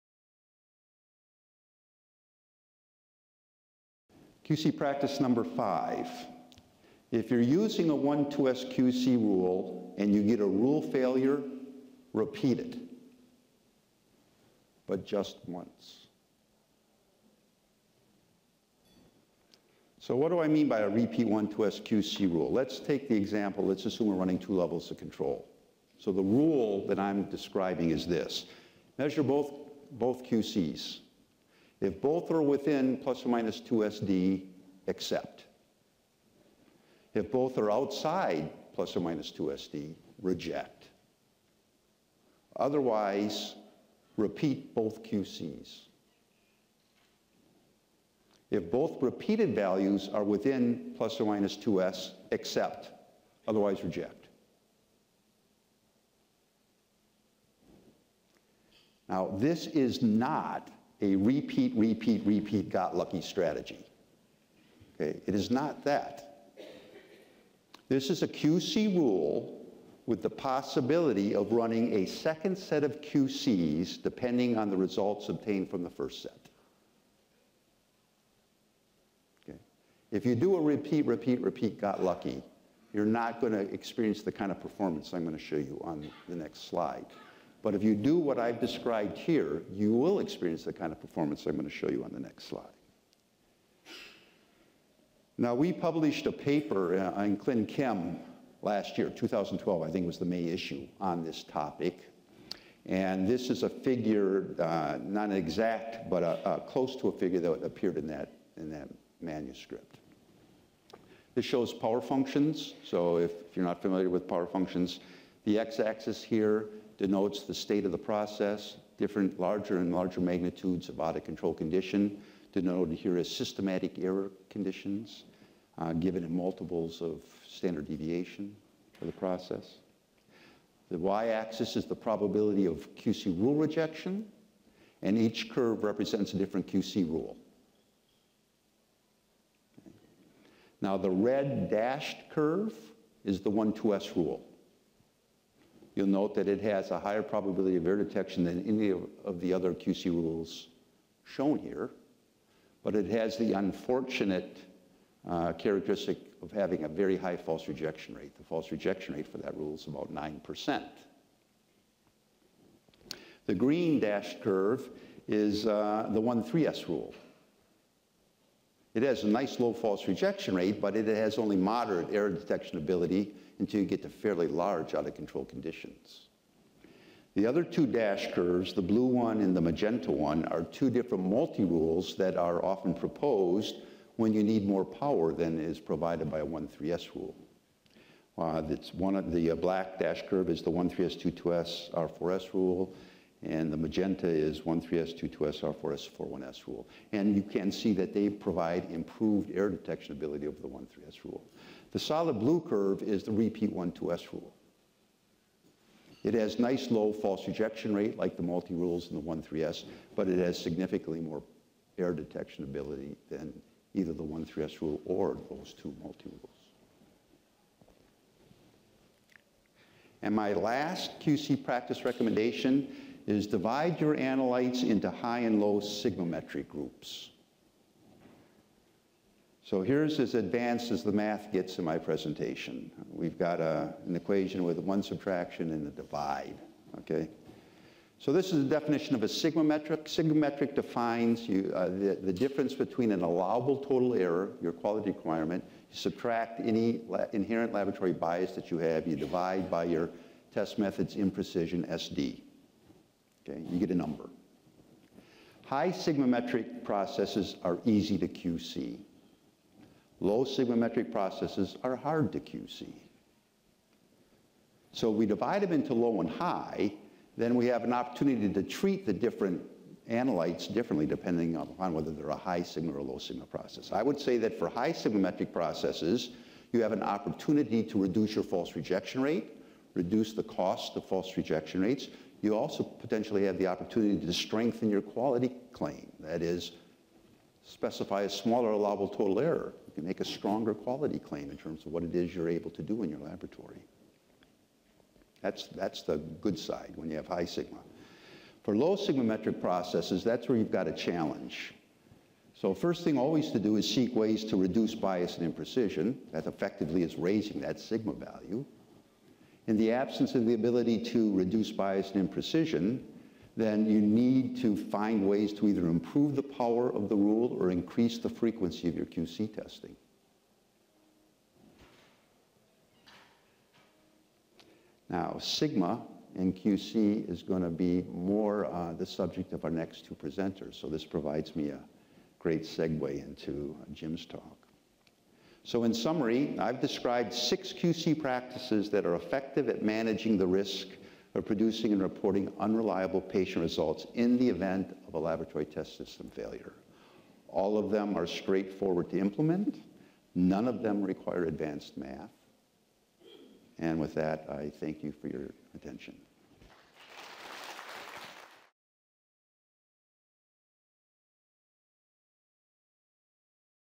QC Rules and Sigma Metrics (Basic)
Curtis Parvin, PhD  Manager of Advanced Statistical Research for  Bio-Rad Laboratories' Quality Systems Division